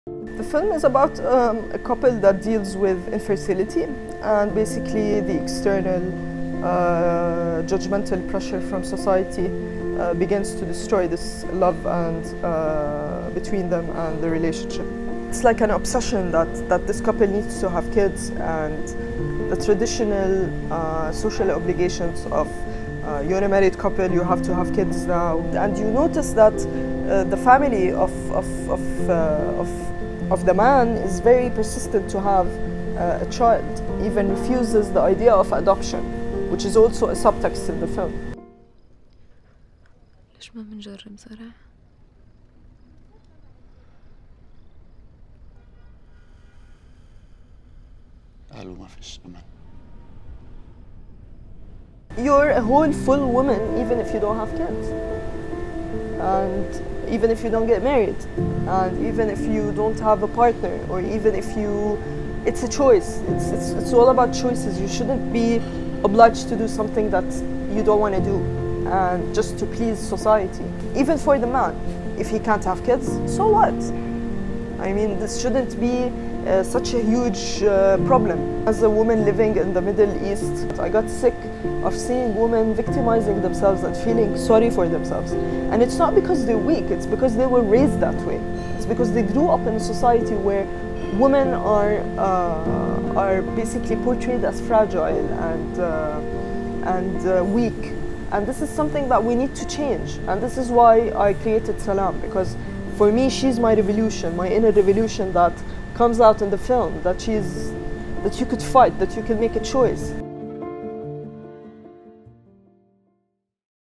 Director Zain Duraie talks Give Up The Ghost, Best Arab Short Film at El Gouna Film Festival
Jordanian film director Zain Duraie talks the story behind Give Up The Ghost (2019), which was awarded 'Best Arab Short Film' during the third edition of the El Gouna Film Festival. Read the full article